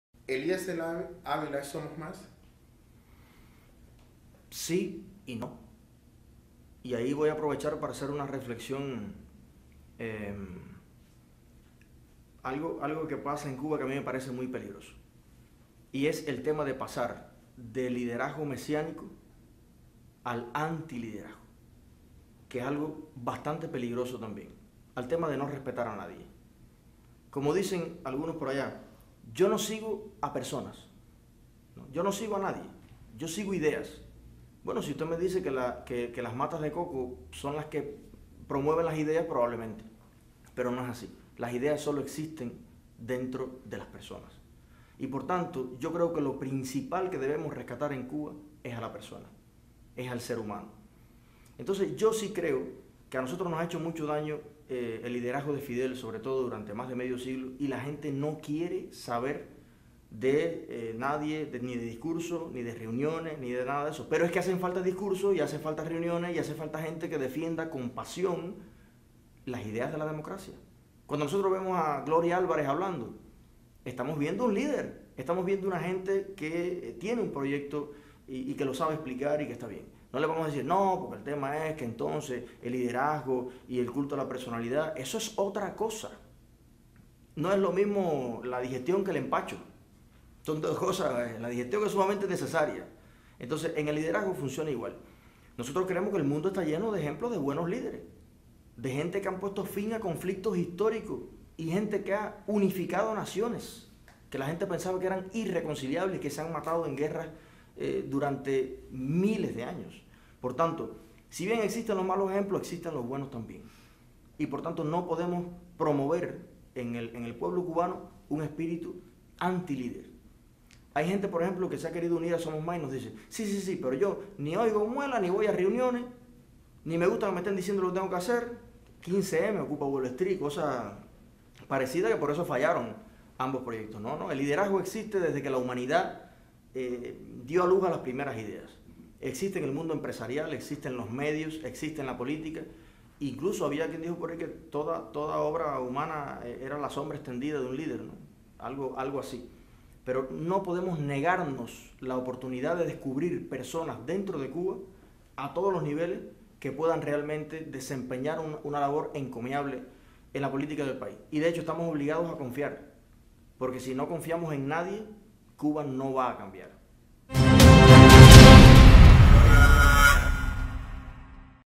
Si no confiamos en nadie, Cuba no va a cambiar. Entrevista a Cubanos por el Mundo.
Movimiento SOMOS+ Es una convicción. Que expresa la seguridad absoluta de que somos mayoría dentro y fuera de Cuba los que no deseamos seguir viviendo bajo las actuales circunstancias políticas y económicas de nuestro país